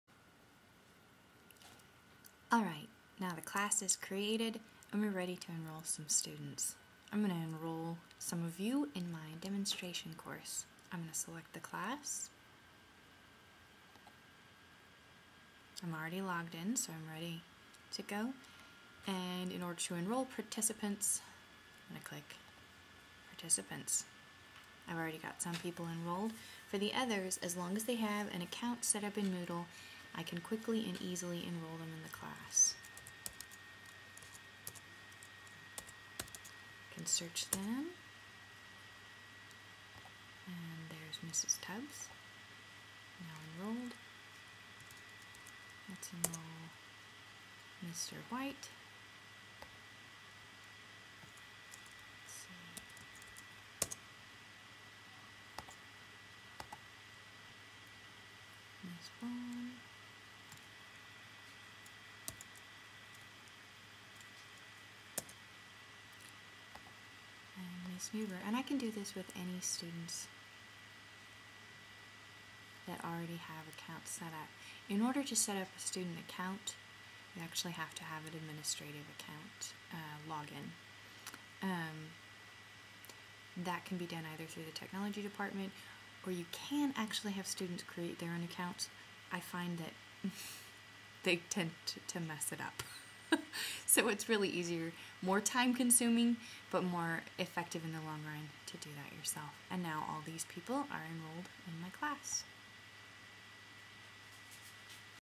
How to Enroll Students in a Moodle Course.mov
This is a brief & basic tutorial for enrolling students/participants in moodle courses. This tutorial was created for participants in the Carthage R-9 PD Workshop, Jan. 19, 2010.